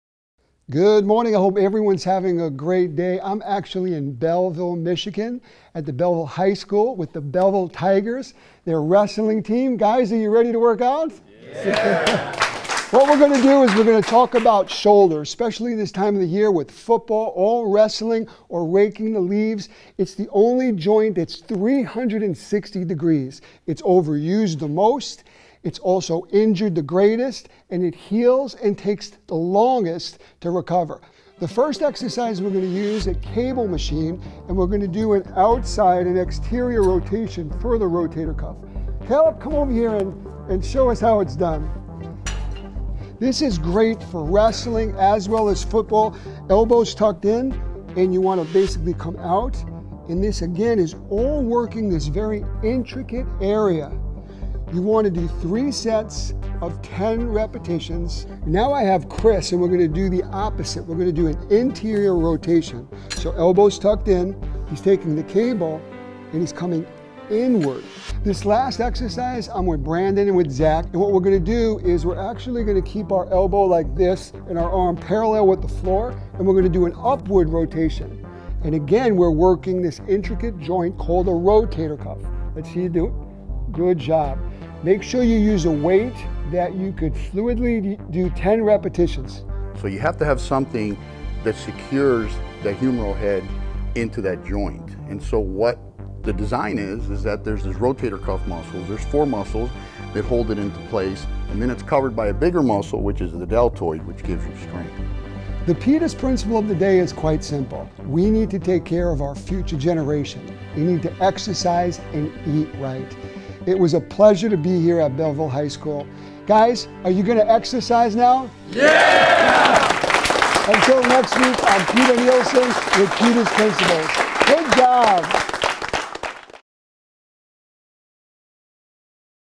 CONDITIONING THE SHOULDERS
Health expert Peter Nielsen explains to high school wrestlers the importance of strengthening the rotator cuff and shoulders.